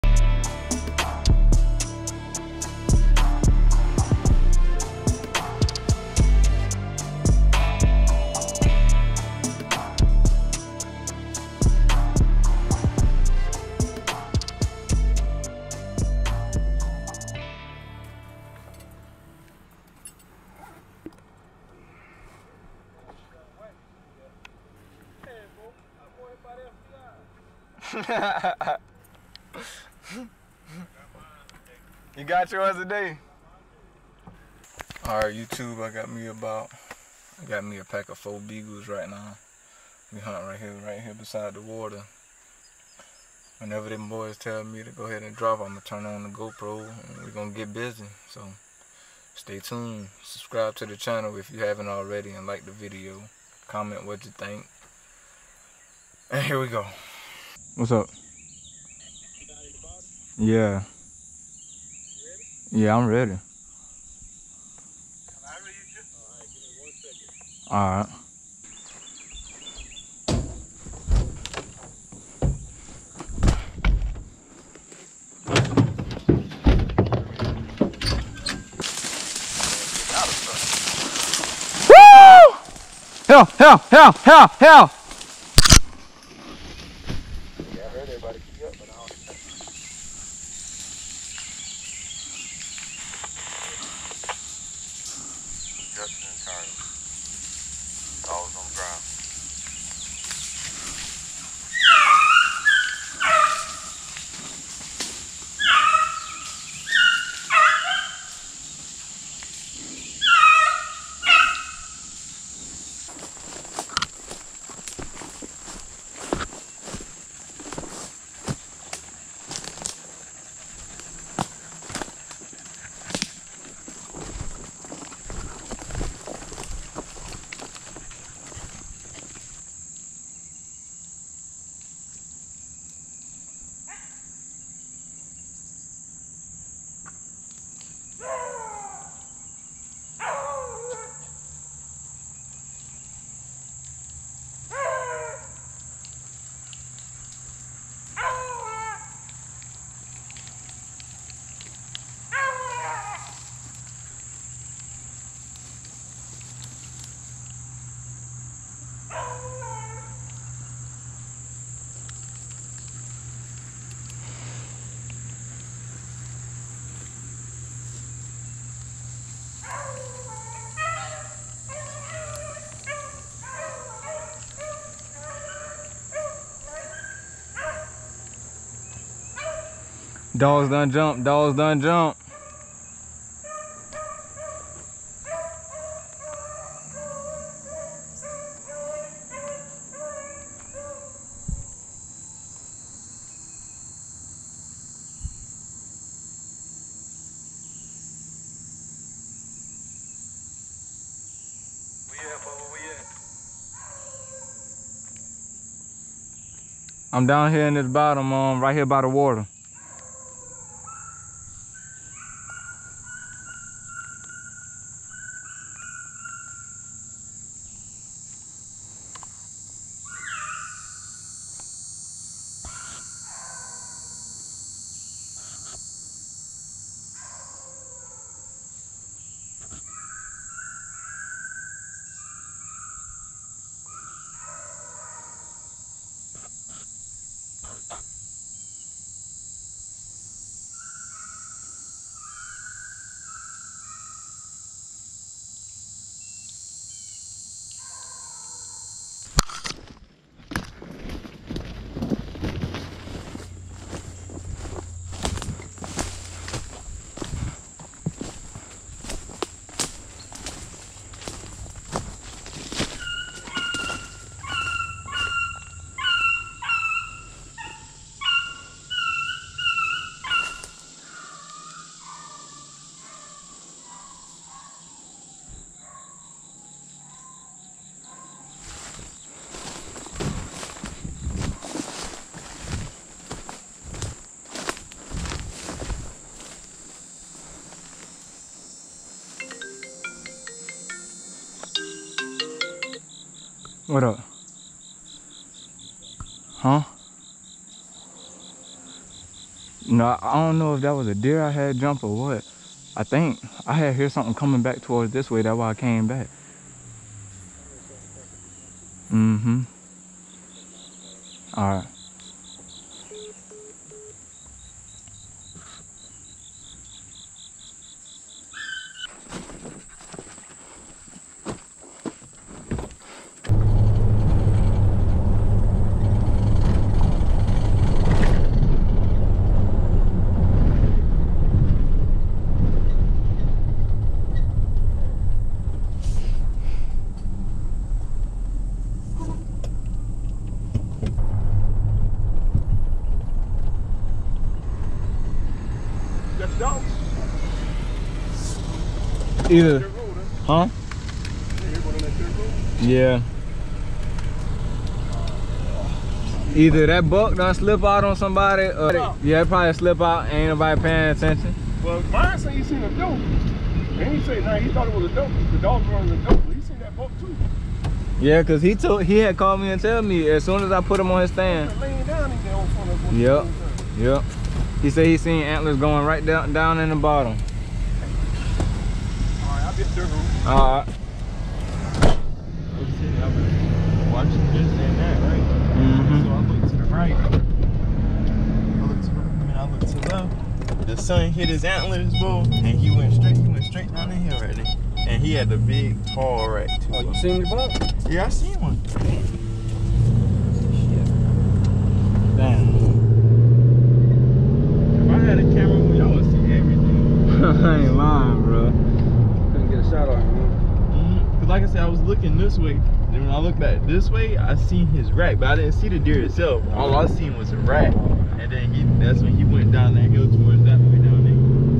Deer Hunting w/ Dogs - Playing A Game of Chess with a Big Buck
On this hunt, we hunted a block of woods in Santee, SC. I brought a pack of four beagles to see if we could get a big buck up out the woods. They jumped him up and then shut off. One of the standers got a good glimpse of the deer's antlers, and it appeared to be a 8-pointer or better.